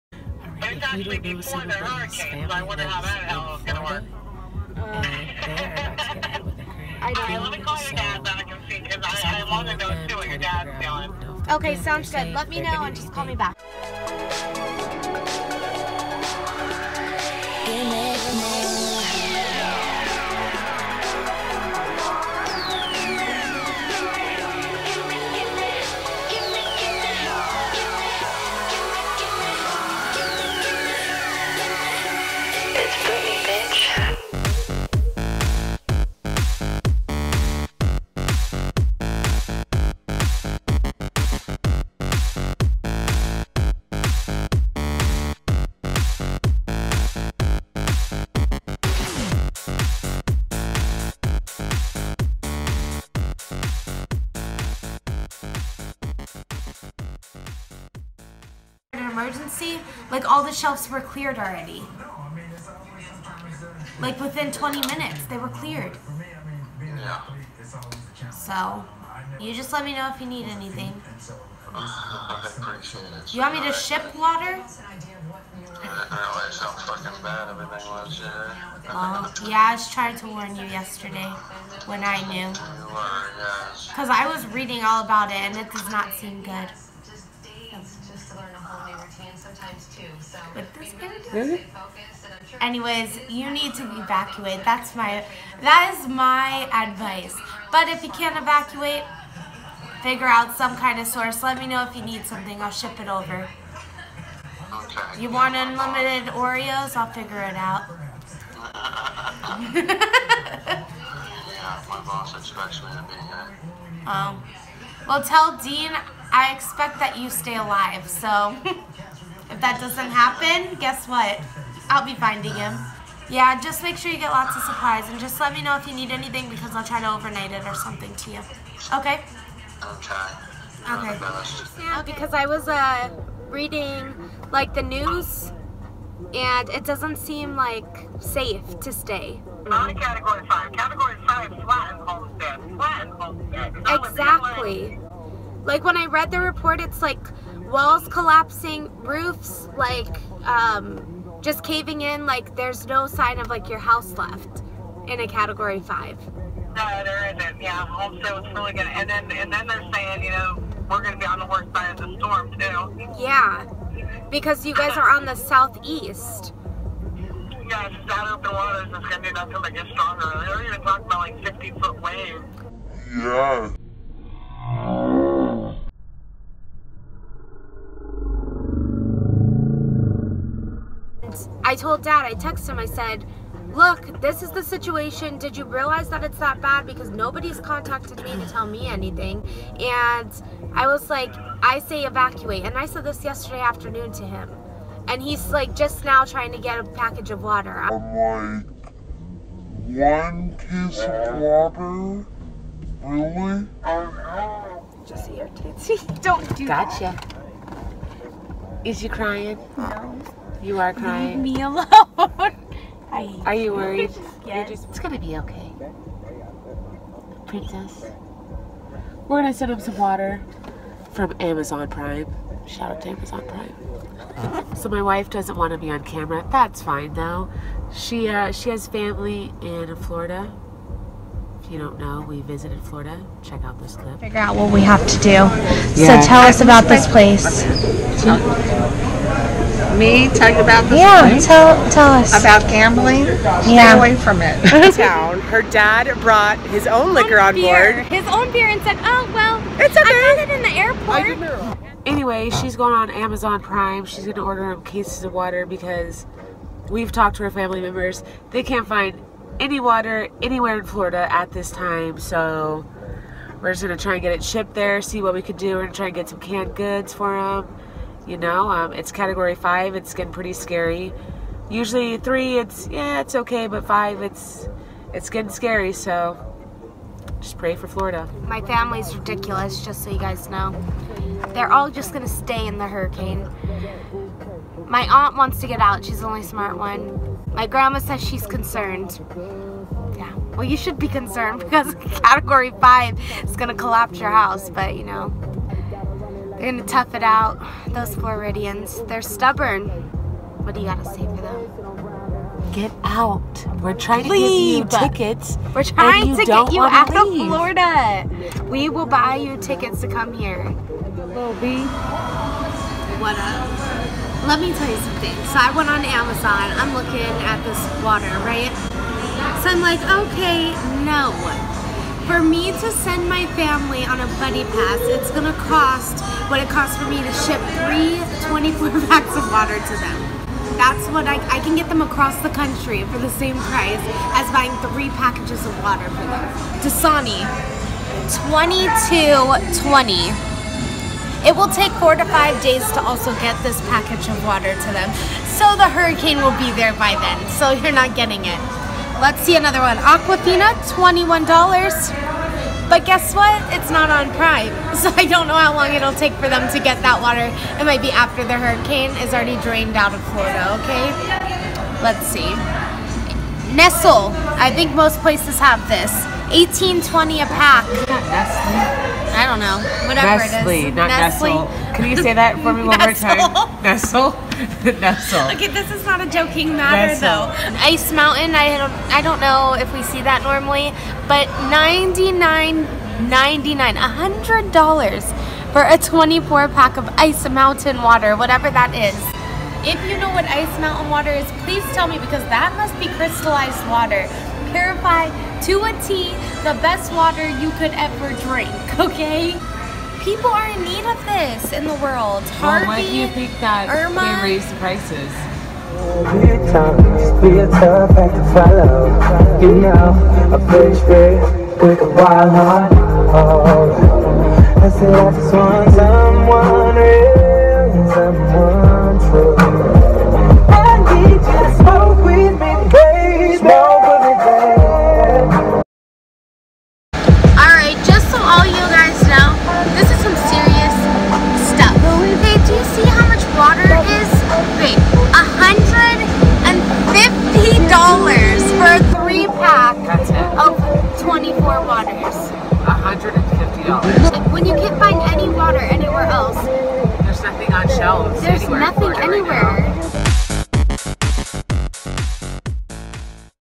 Flight attendant| Hurricane IRMA
Our family is stranded in Florida and the water supply has run out! Flights are booked to full capacity and Floridians are being told to evacuate due to Hurricane Irma. This is a real day in the life of The Brittanys.